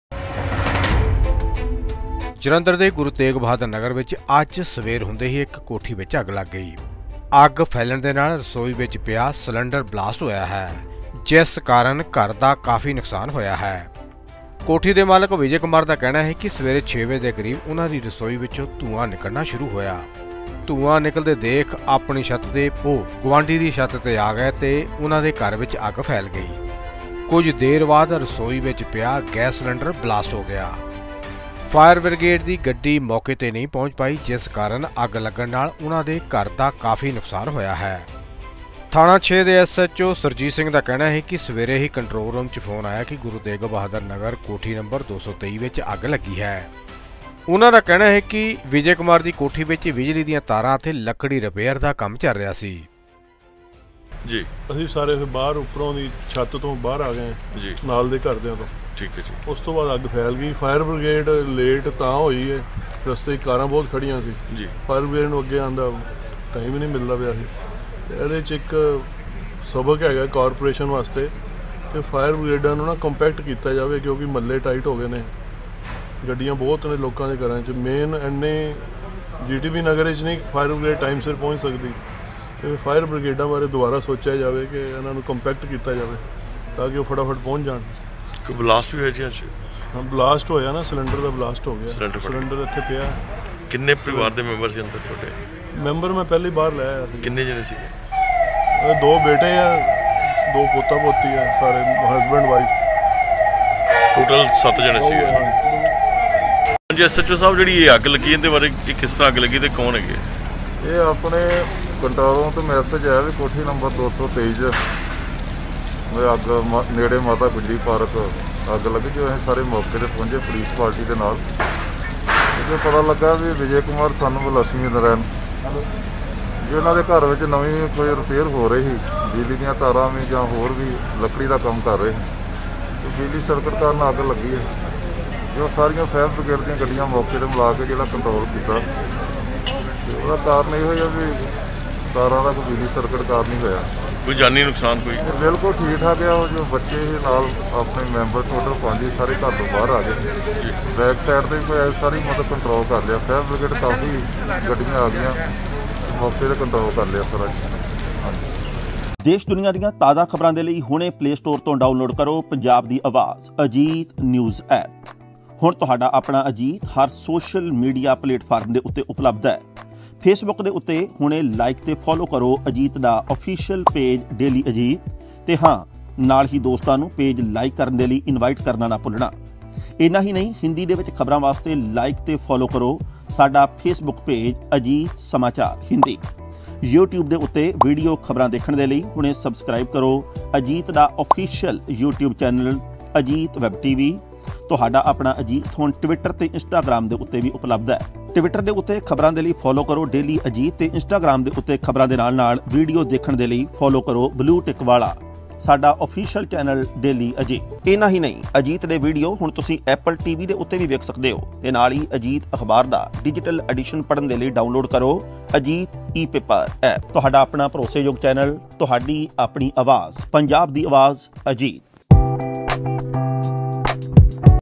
ਜਲੰਧਰ : ਕੋਠੀ 'ਚ ਲੱਗੀ ਭਿਆਨਕ ਅੱਗ
ਅੱਗ ਰਸੋਈ 'ਚ ਫੈਲਣ ਤੋਂ ਬਾਅਦ ਸਲੰਡਰ 'ਚ ਹੋਇਆ ਬਲਾਸਟ